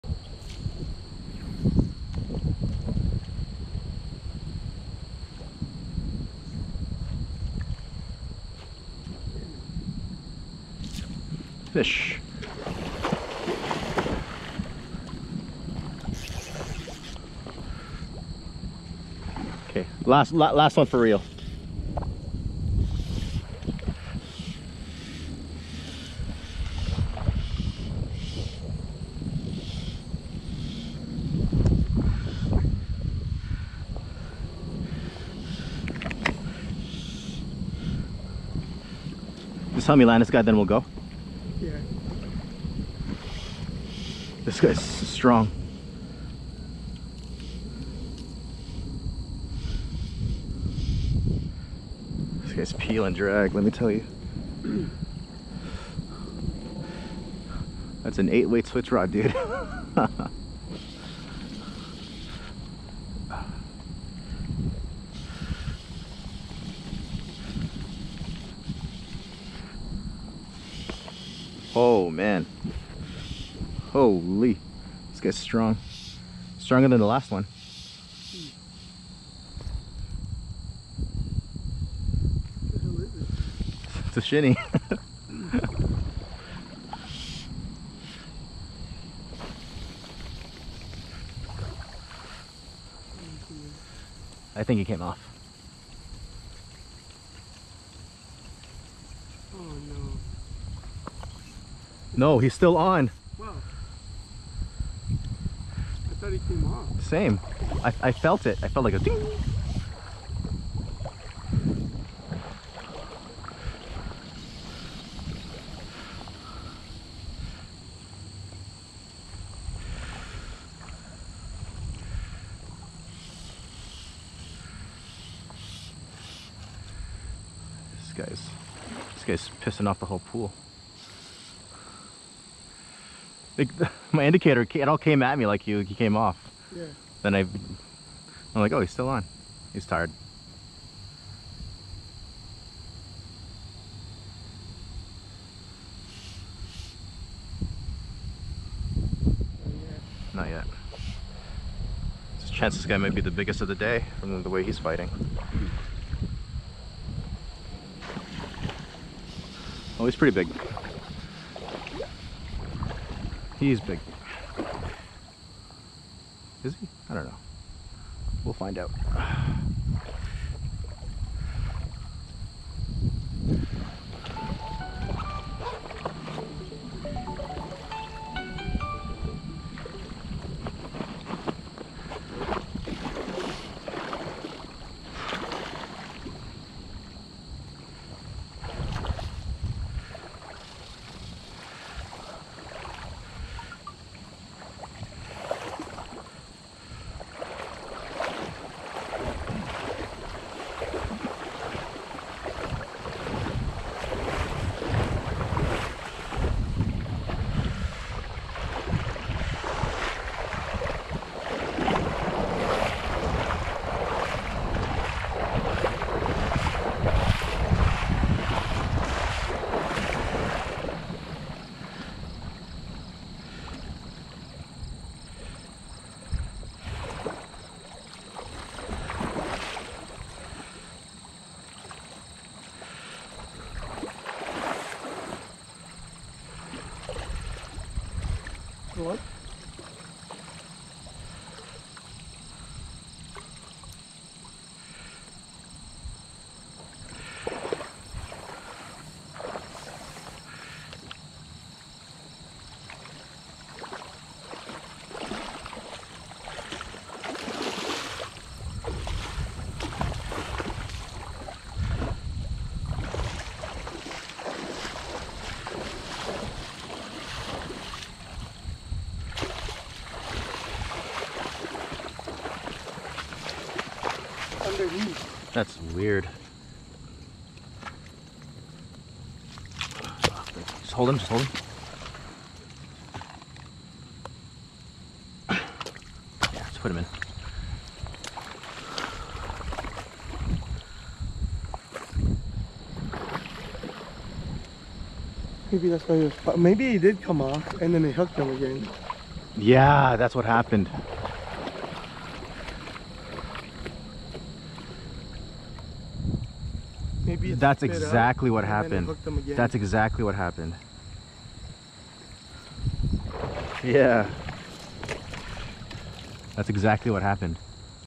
Ontario Salmon/Steelhead: Salmon spits fly, but hooks himself again
This Chinny spits hook at 3:19, but manages to hook himself on the chin.

Rod - Fenwick Aetos, 11'1" 7/8 WT switch rod
Reel - Lamson Remix, 4
Line - Piscifun Sword, WF10F
Butt Section -  Maxima Ultragreen, 20 lb.
Strike Indicator - Air-Lock, 3/4"
Tippet - Maxima Flourocarbon, 10 lb.
Fly/Bait - Stonefly, size 8